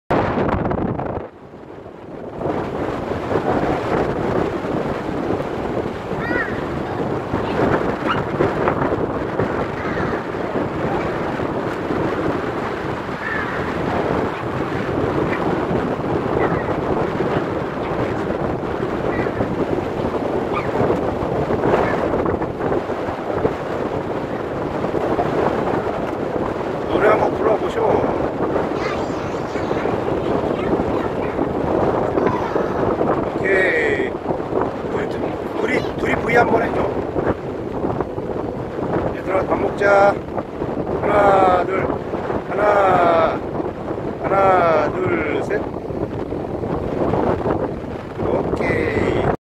강풍속에서 점핑놀이하는 아이들 [2017.05.13]
2017년 5월 13일 토요일...태안군 안면도의 한 해수욕장 옆 항구 갯바위에서 사리물때에 맞추어서 간조전까지 오전해루질을 즐기고 바로 옆에 있는 모래로 된 갯벌에서 마냥 신나게 모래장난하면서 놀고 밀물과 풍랑이 시작되어 민박으로 철수중 강풍속 점프놀이를 하는 아이들...역시, 아이들은 자연에 몸을 맡긴채 신나게 놀아야 스트레스가 팍팍 풀리는듯...